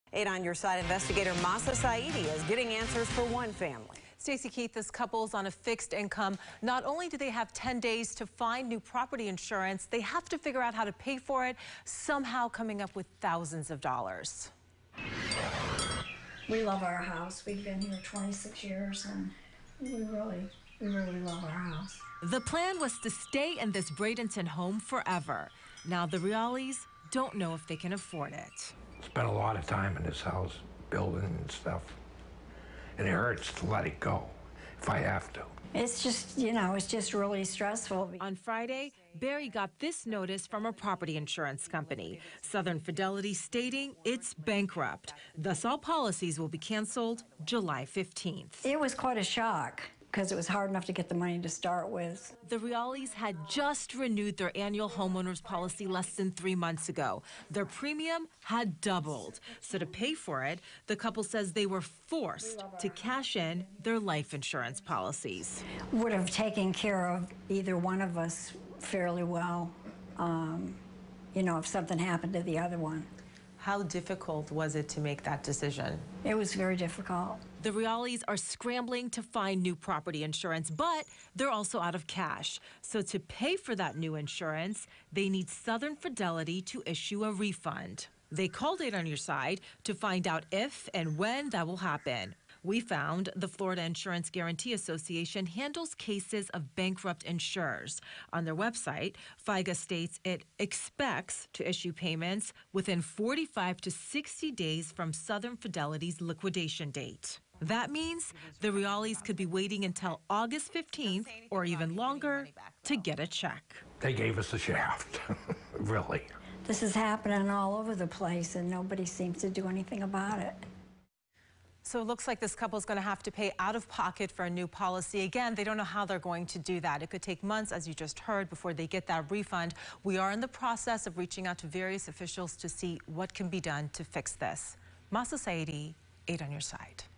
Southern Fidelity Property Insurance bankrupt: When can you expect premium refund?
8 On Your Side Investigator Mahsa Saeidi filed this report at 6pm.